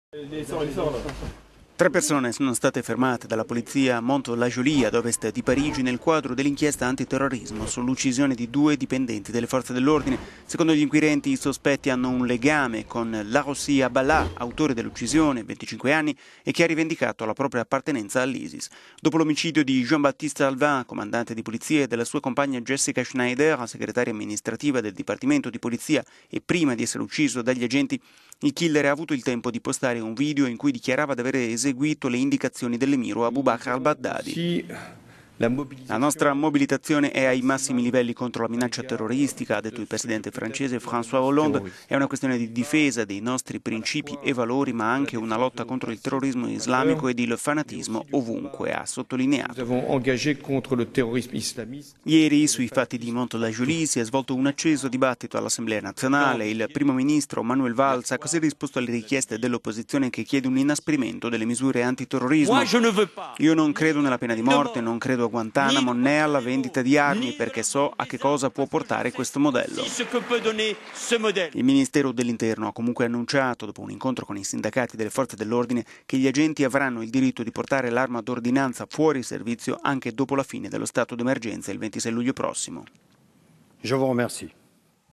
Francia, fermati 3 presunti complici del killer dei poliziotti
Tre persone sono state fermate dalla polizia a Mantes-la-Jolie, ad Ovest di Parigi, nel quadro dell'inchiesta antiterrorismo sull'omicidio di due dipendenti delle forze dell'ordine.

Secondo gli inquirenti i sospetti hanno un legame con Larossi Abballa, autore dell'uccisione, 25 anni, e che ha rivendicato la propria appartenenza all'Isis.

Dopo l'omicidio di Jean-Baptiste Salvaing, 42 anni, comandante di polizia, e della sua compagna Jessica Schneider, 36 anni, segretaria amministrativa del dipa…